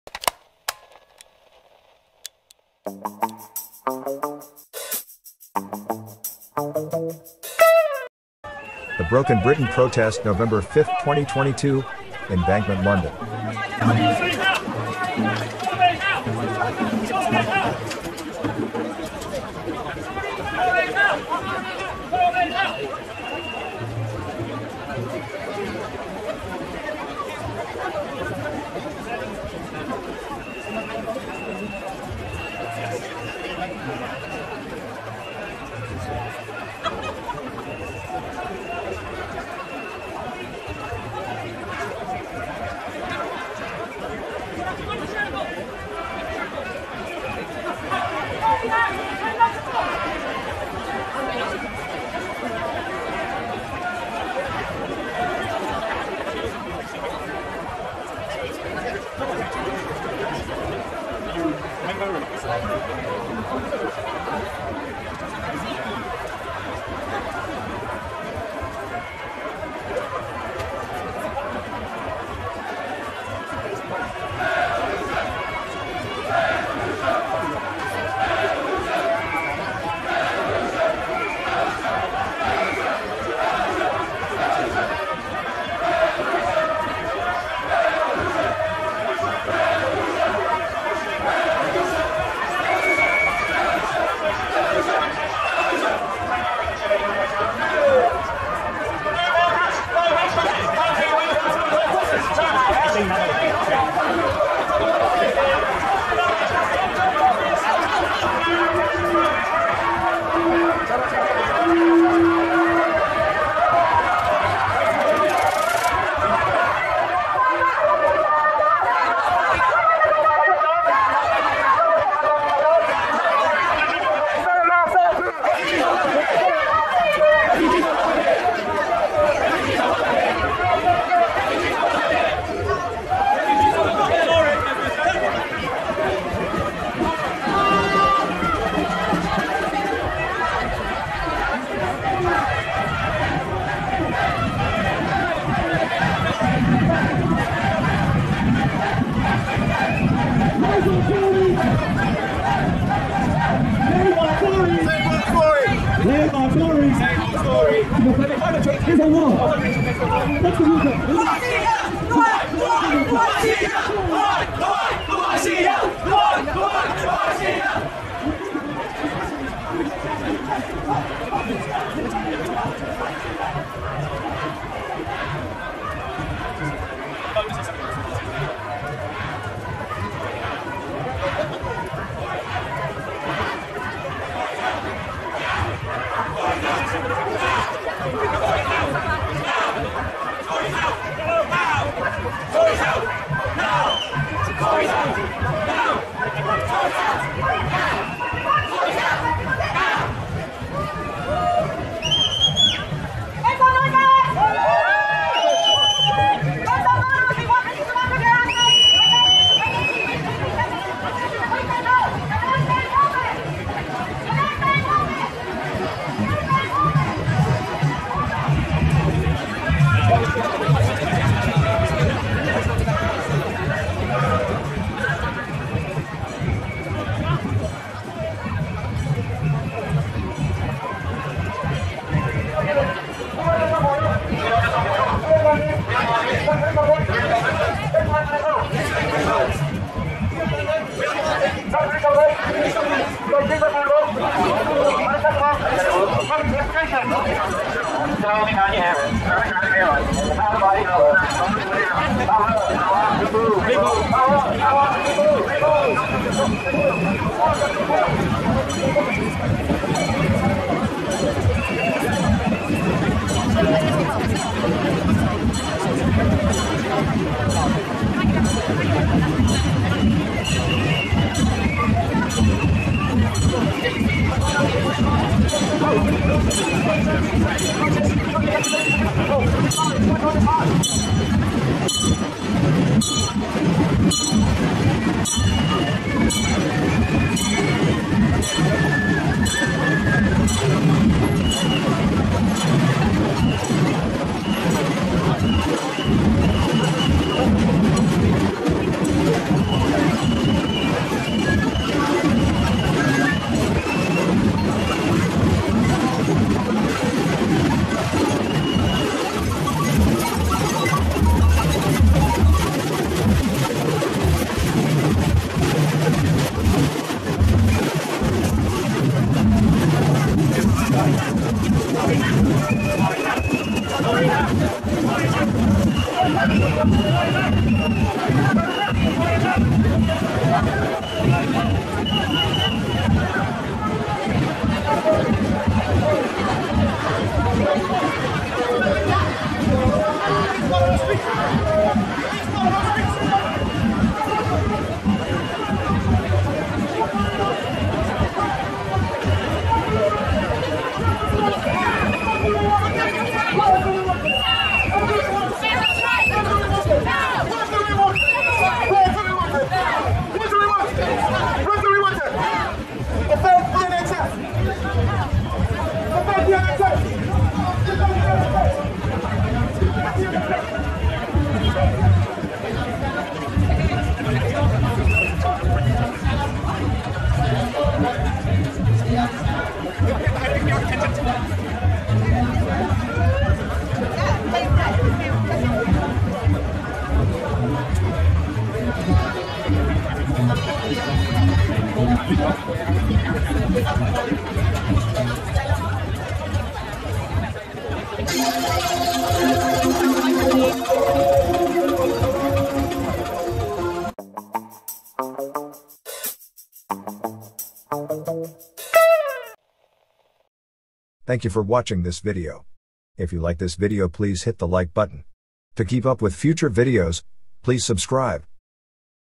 BROKEN BRITAIN PROTEST EMBANKMENT LONDON
NEWS  S03:E38
BRKEN BRITAIN PROTEST EMBANKMENT LONDON AS THE MARCH TAKES OFF - WE STAND IN ONE SPOT AND RECORD PASSING US.